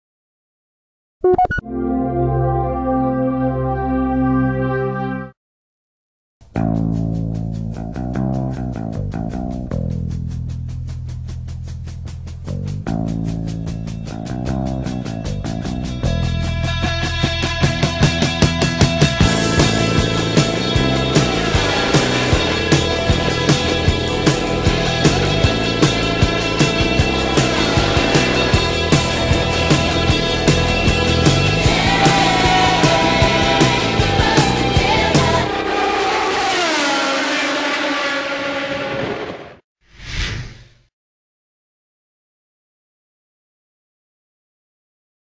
S1 Grand Prix - Launch Teaser
Smile Foundation and Tyremen presents 'S1 Grand Prix'.
Launch date: 11 April 2013.
Venue: Preston Road, Hull.